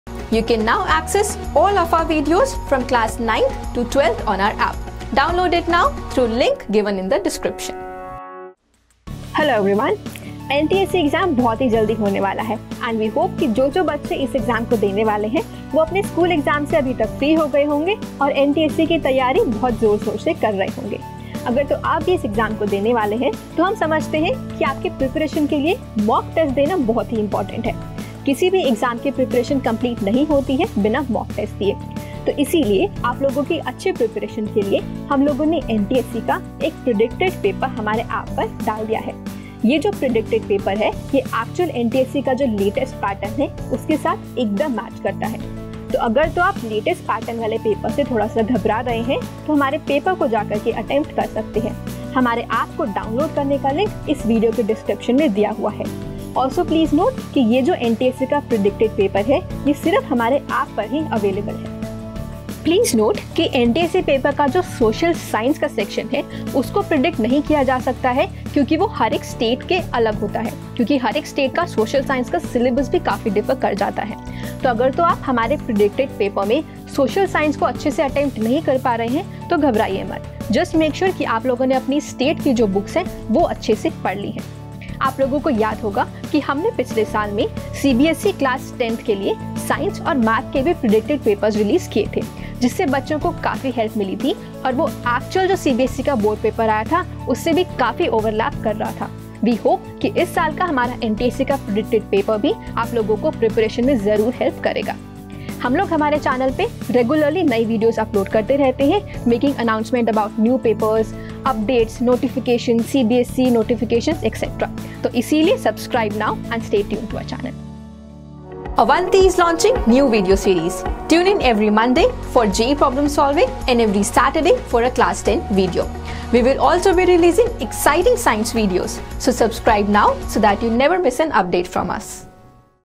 NTSE 2018 PREDICTED PAPER | जानिए कैसा होगा NTSE का पेपर
NTSE 2018 Stage 1 is almost here and to make the exam easier for you, we have created a predicted paper / guess paper exclusively for the users of our app.

This paper contains the following subjects -
- MAT
- SAT: Biology, Physics, Chemistry, Maths, History, Civics, Geography, Economics

Note that just like in the actual exam, there are two separate papers on the app - one for MAT and one for SAT.